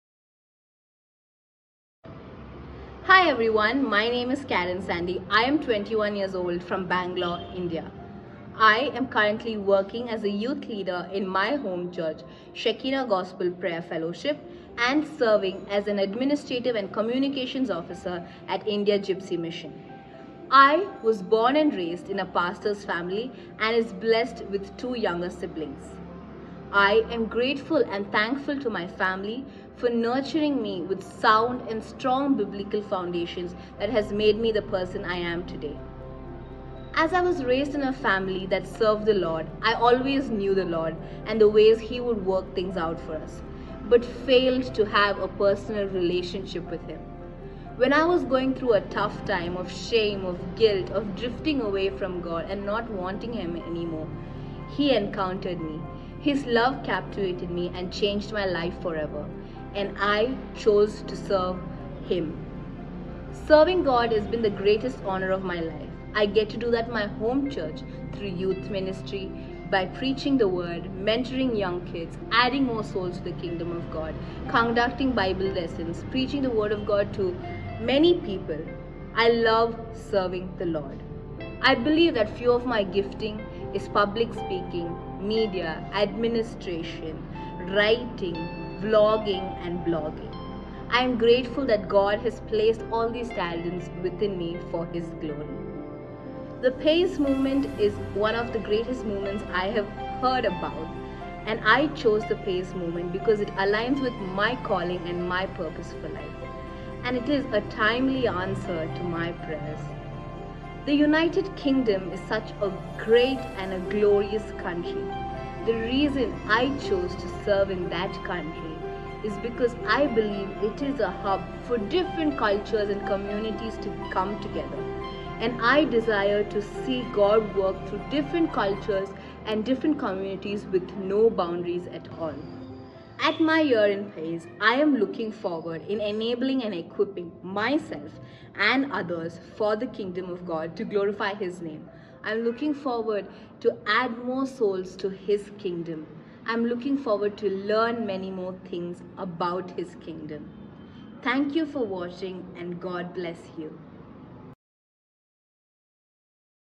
Bethany Church - Introducing Karen Sandhy - New addition to our fabulous Pais Youth Team
A Bible based pentecostal church based in South Manchester UK